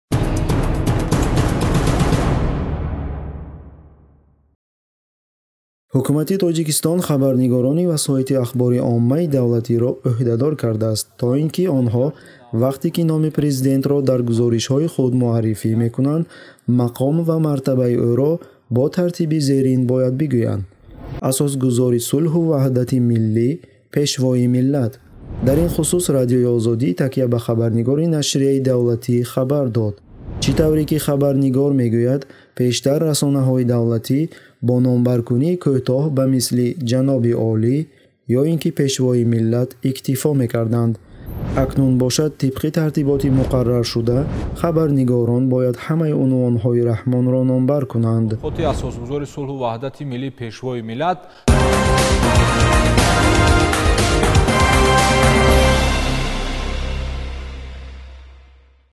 Тартиботи нави муроҷиат ба президент Раҳмон
Ҳукумати Тоҷикистон хабарнигорони Васоити Ахбори Оммаи давлатиро ӯҳдадор кардааст, то ин ки онҳо вақте ки номи президентро дар гузоришҳои худ муаррифи мекунанд, мақом ва мартабаи ӯро бо тартиби зерин бояд бигӯянд: "Асосгузори сулҳу ваҳдати миллӣ - Пешвои миллат".